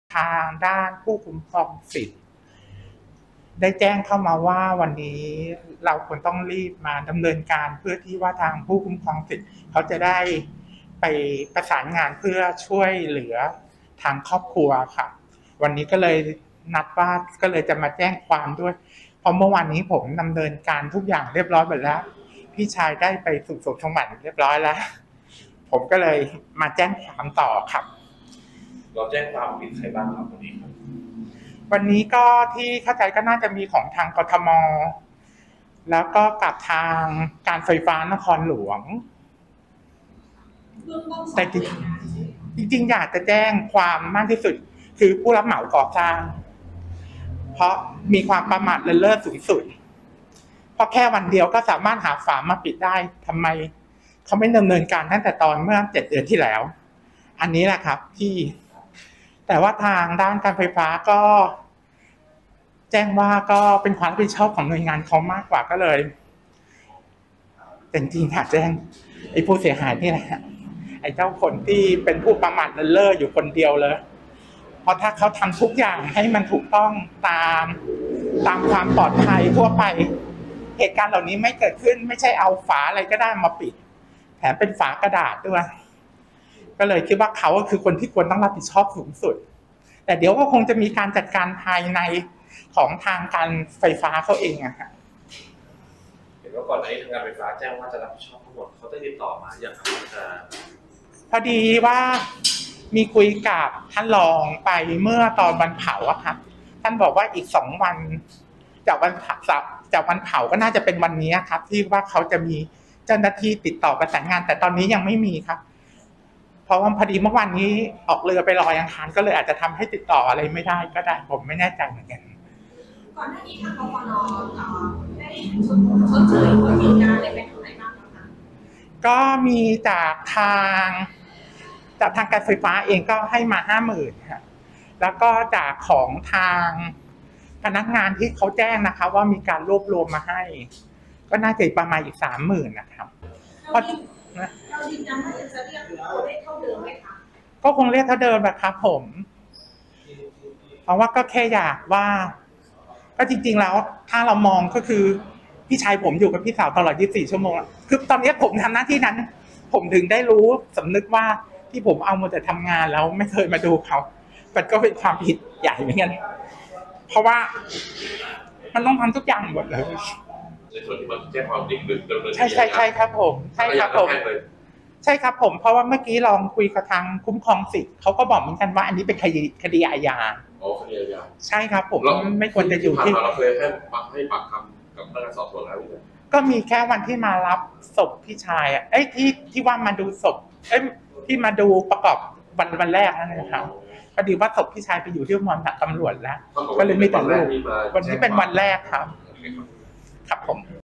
ครอบครัวลุงตกท่อดับ แจ้งเอาผิด กทม.-กฟน. ยันเรียกค่าเสียหาย 10 ล้าน
เจ้าหน้าที่กรมคุ้มครองสิทธิฯ พาครอบครัวของชายตกท่อร้อยสายไฟเกาะกลางถนนลาดพร้าวเสียชีวิต เข้าแจ้งความตำรวจ สน.โชคชัย เอาผิด กทม.-กฟน. ยืนยันเรียกค่าเสียหาย 10 ล้านบาท